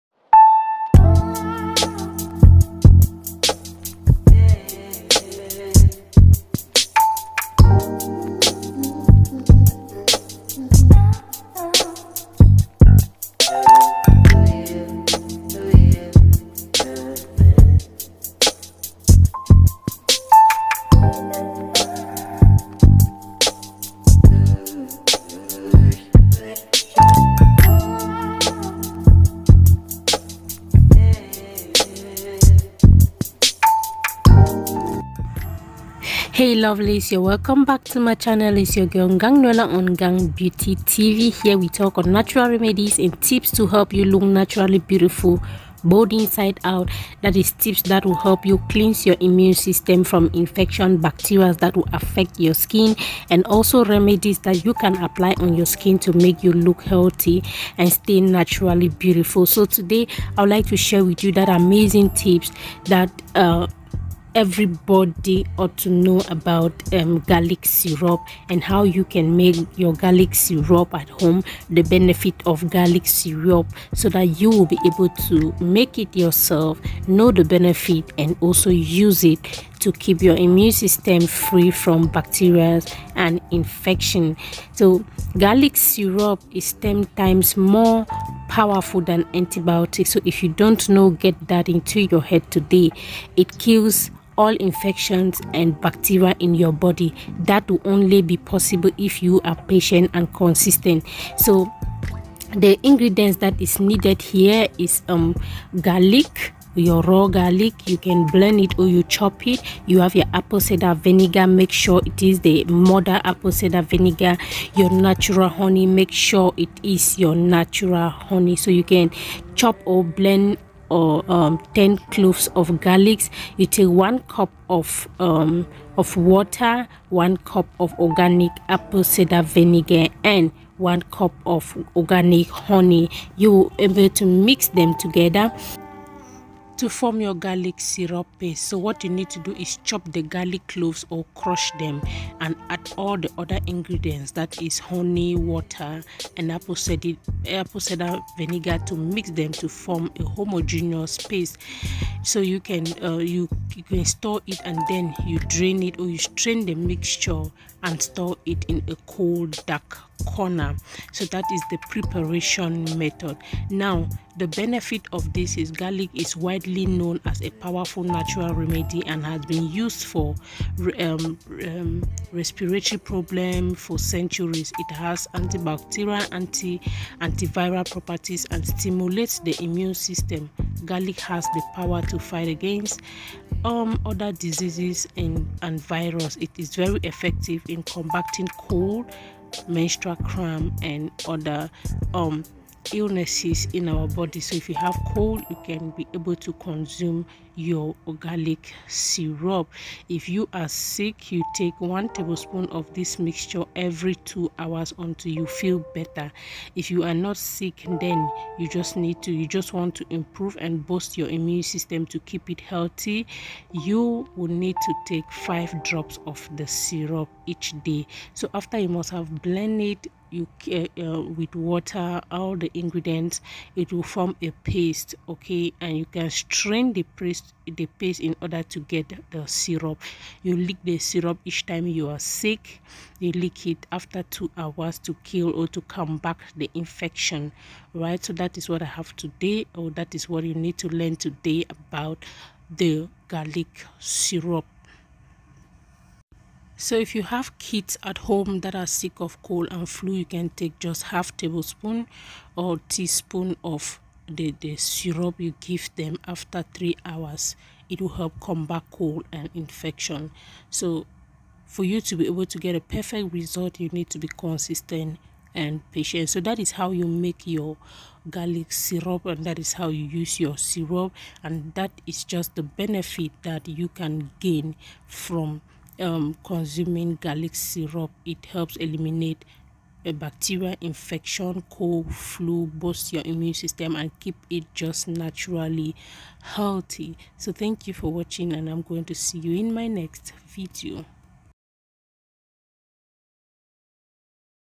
What everybody ought to know about garlic syrup,How to make garlic syrup,life-saving tips
WHAT EVERYBODY OUGHT TO KNOW ABOUT GARLIC SYRUP, LIFE-SAVING TIPS .
JUNE

viste my websites